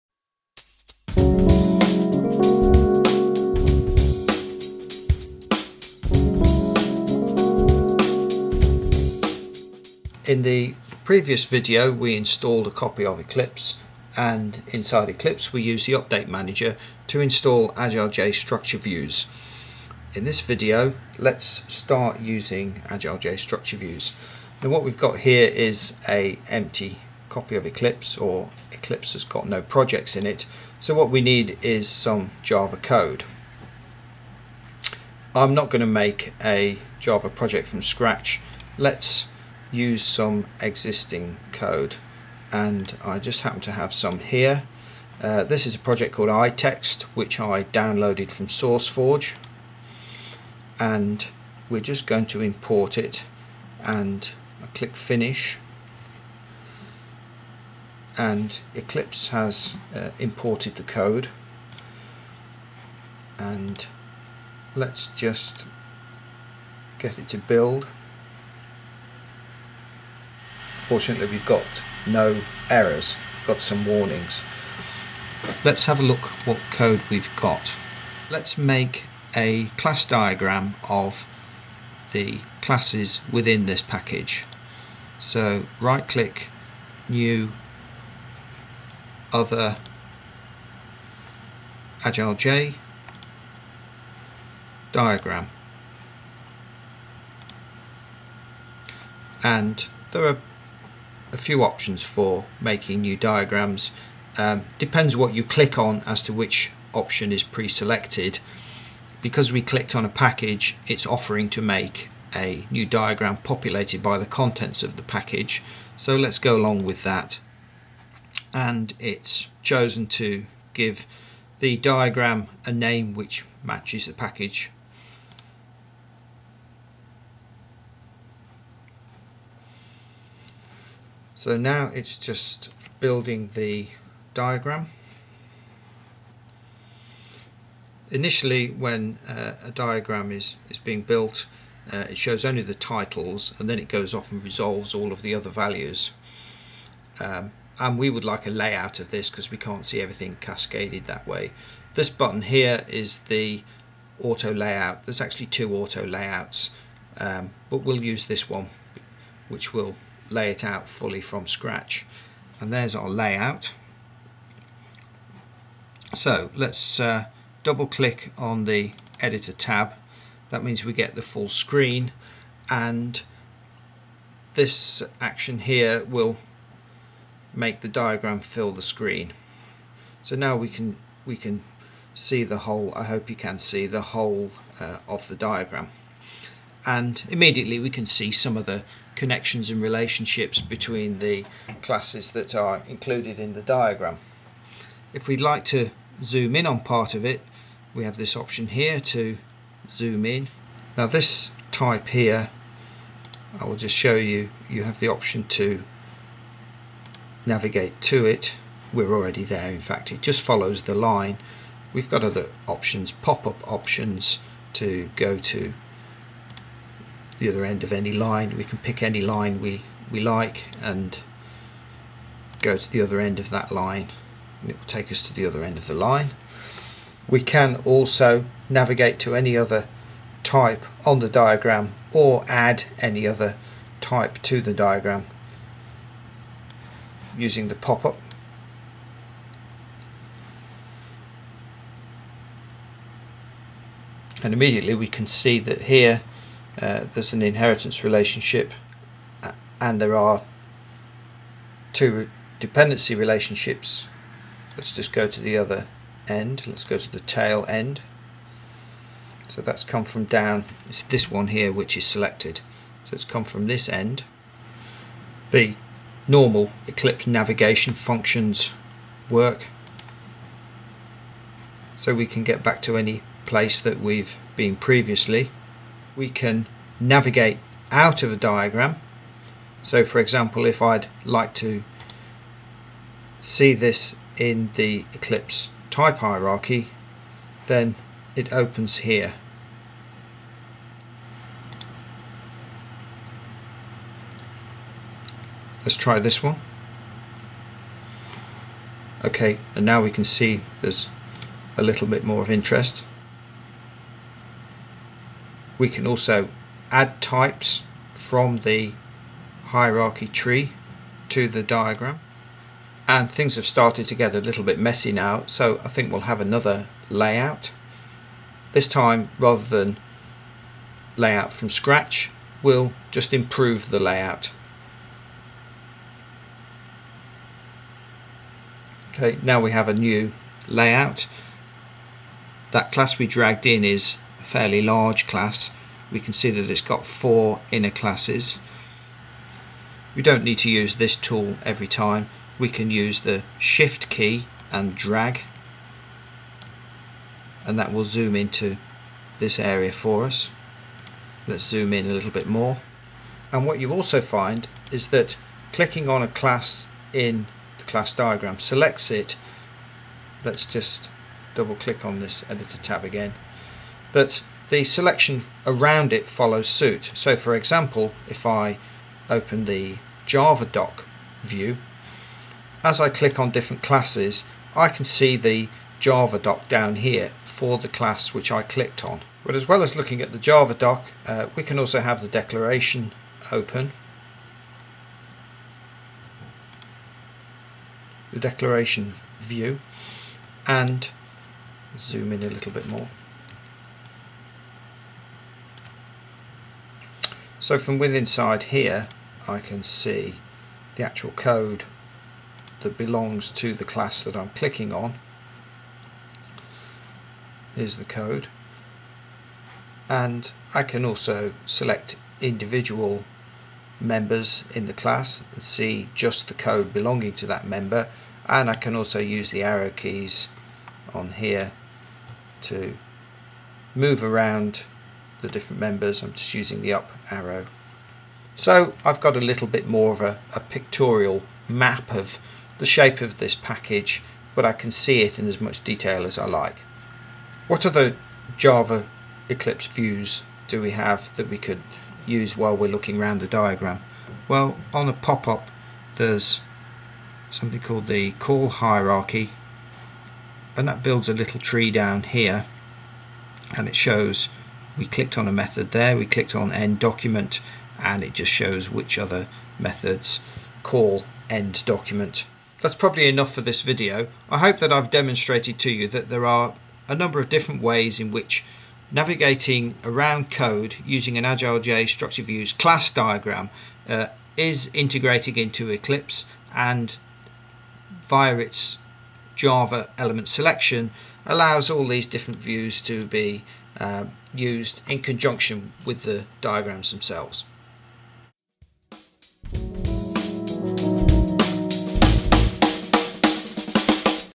An Eclipse Java Navigation Hub
Class diagrams are a powerful way of browsing existing Java code. This videos shows how to build a class diagram of a single package, navigate within the diagram, navigate out of the diagram to the Eclipse type hierarchy then use Eclipse Java browsing views to inspect the code using the diagram as a navigation hub.